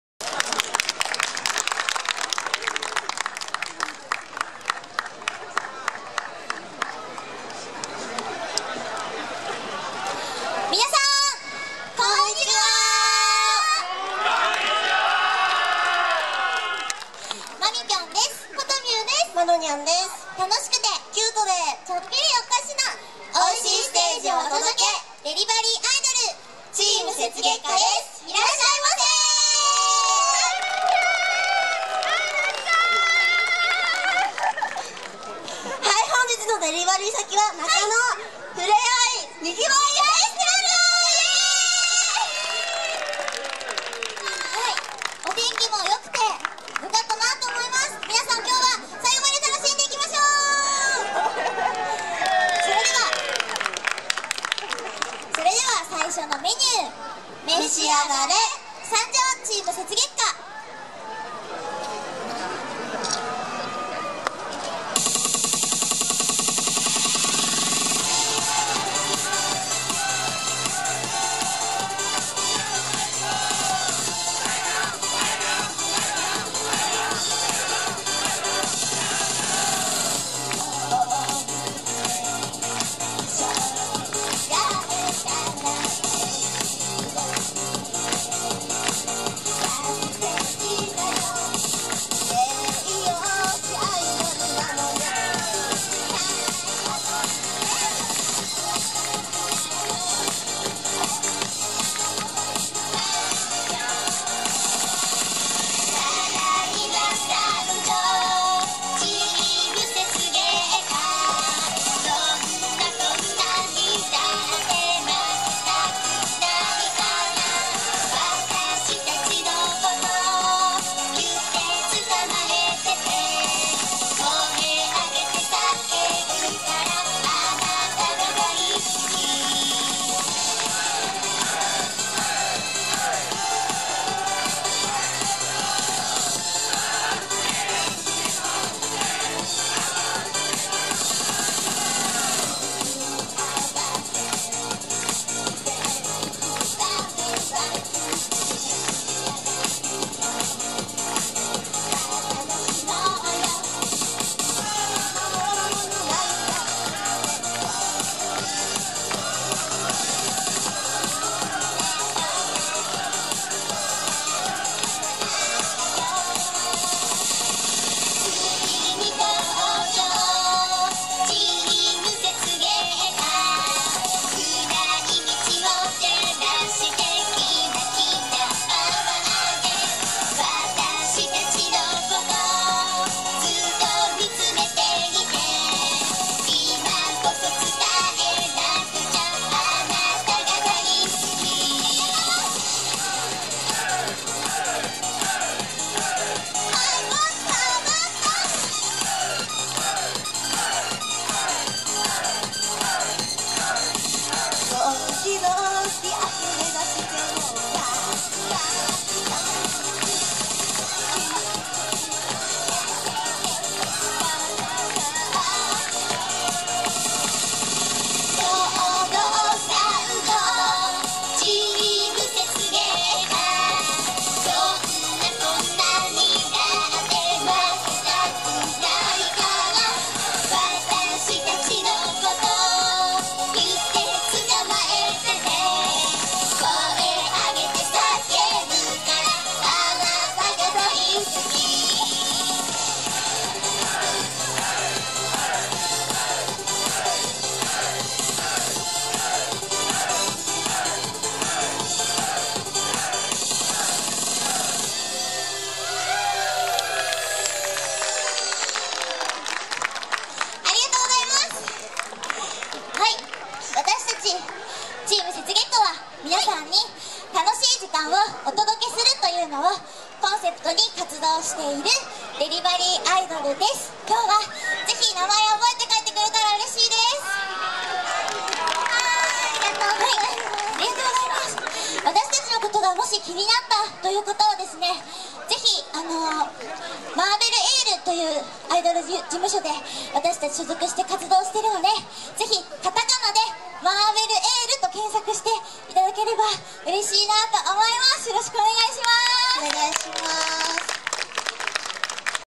Silent Assistant in Japan - First Serving
I can't remember the name of the idol group, but they were fun to watch.